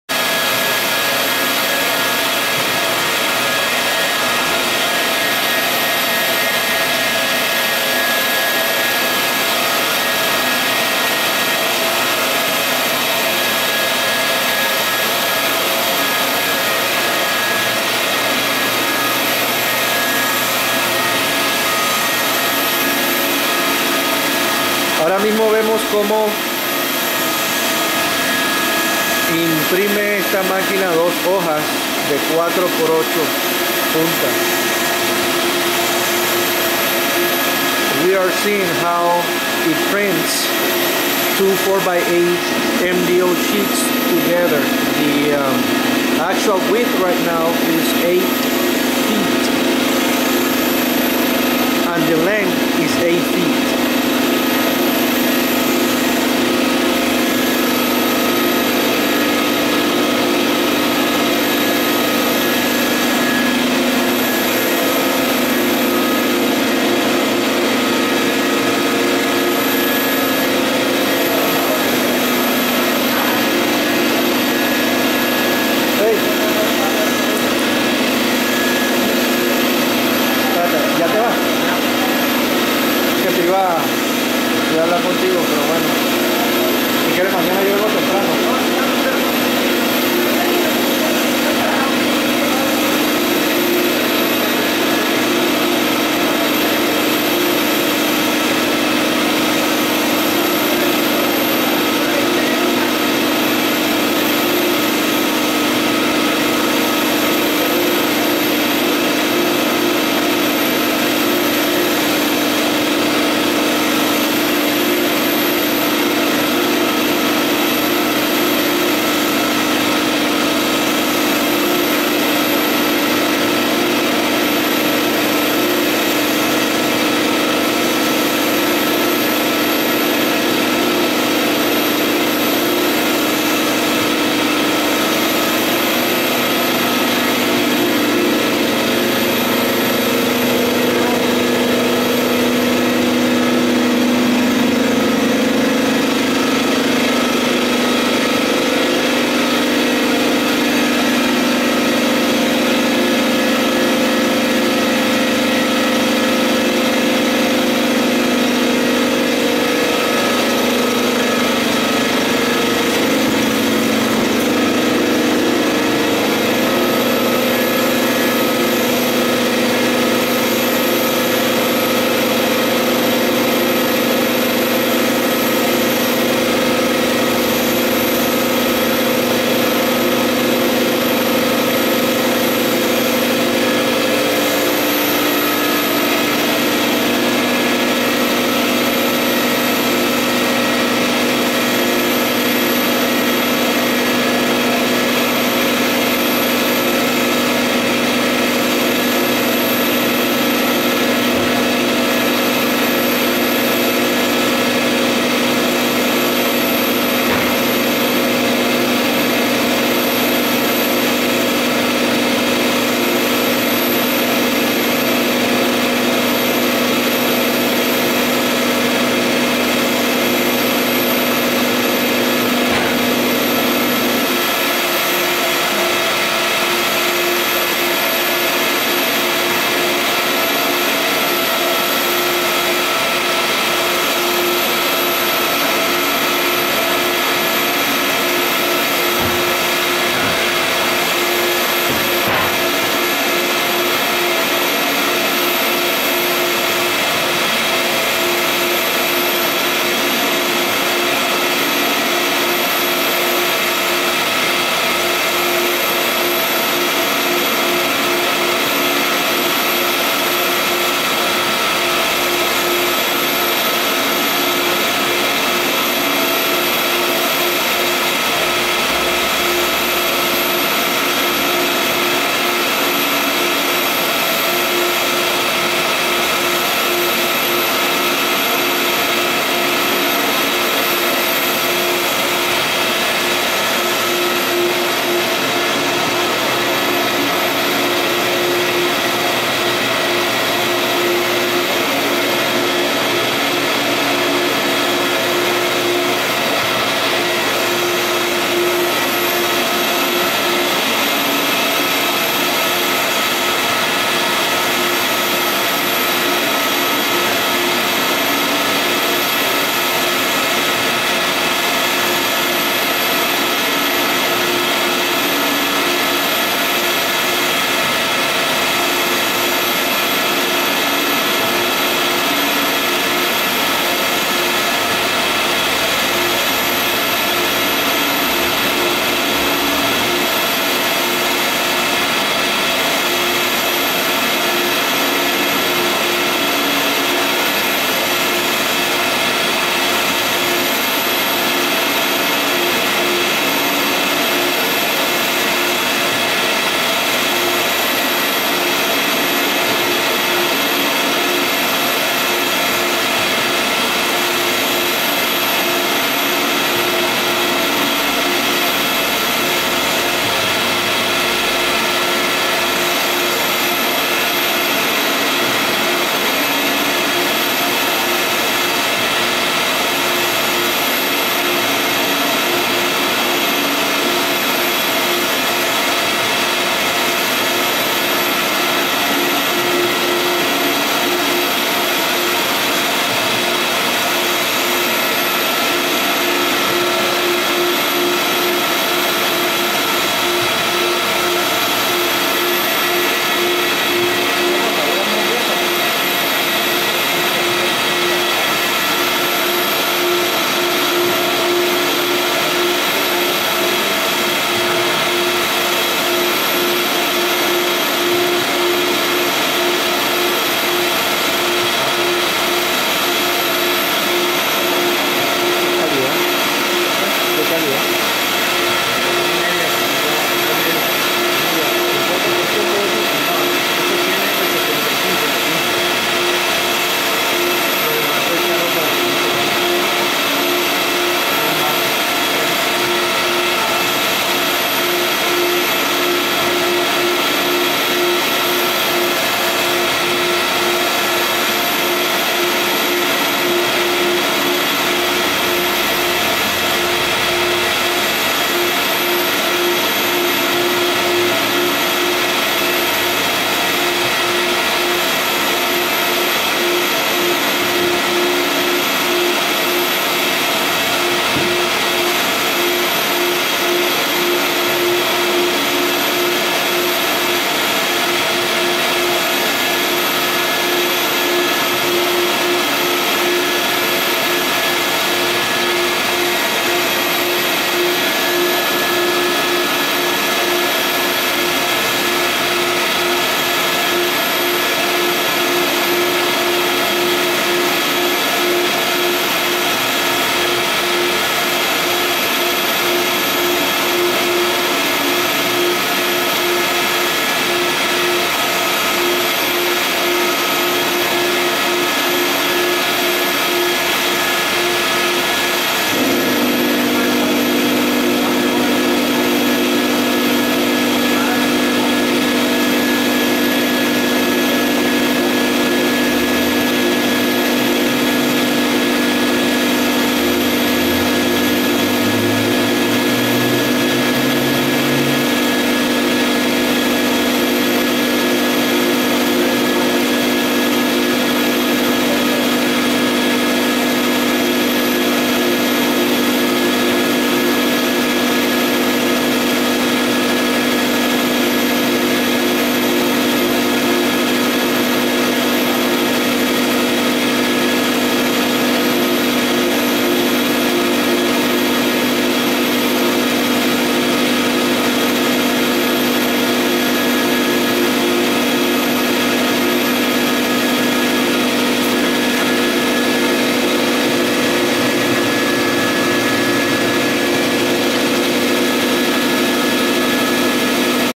Print Factory Vutek Printers.MPG
Print Factory's state of the art technology, whit its Vutek and Roland printers up to 10.5 ft wide and capability to print directly on rigid substrates; Print Factory can manage any outdoor advertising, retail decor or sign project. contact us at /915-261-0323 /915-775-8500 / /